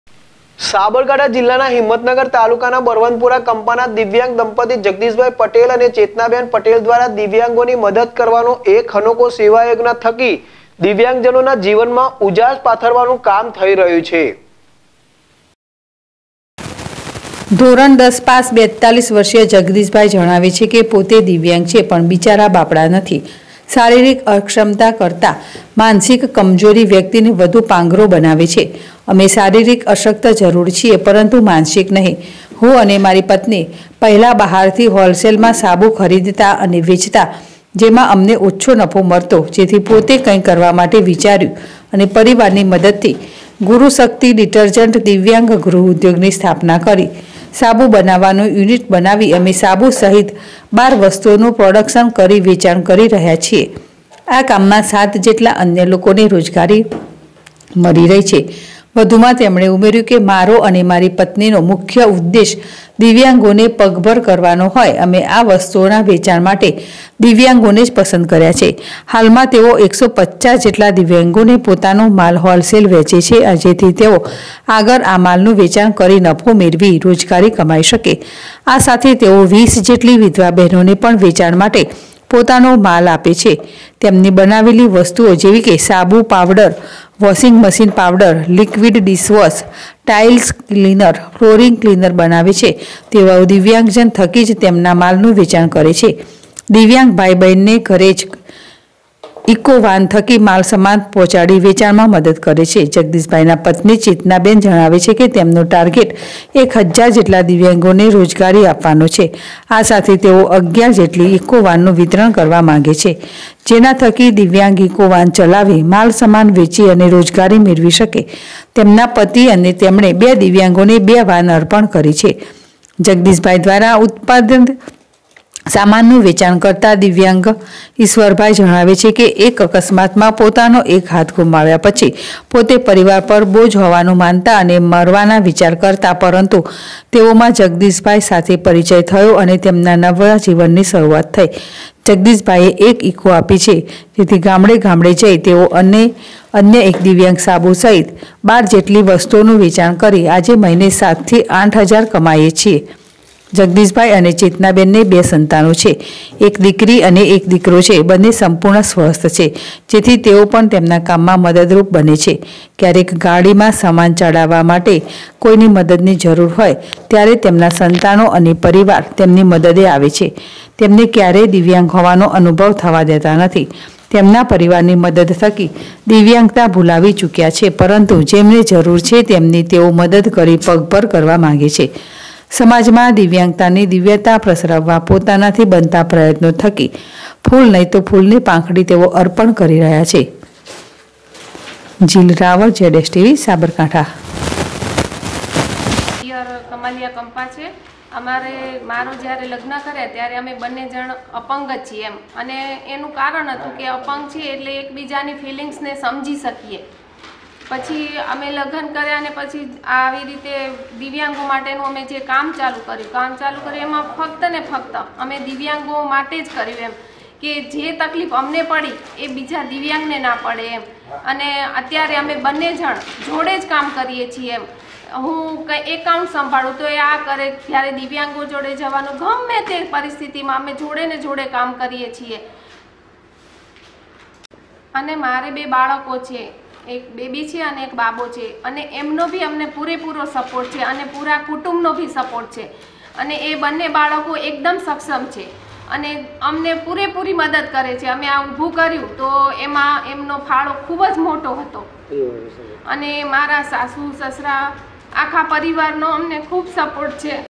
દિવ્યાંગ દંપતીએ અનોખો સેવાજ્ઞન કર્યો શરૂ અનેકો દિવ્યાંગ લોકોને પૂરી પાડે છે રોજગારી.
સાબરકાંઠા જિલ્લા દિવ્યાંગ દંપતીએ અનોખો સેવાજ્ઞન કર્યો શરૂ અનેકો દિવ્યાંગ લોકોને પૂરી પાડે છે રોજગારી.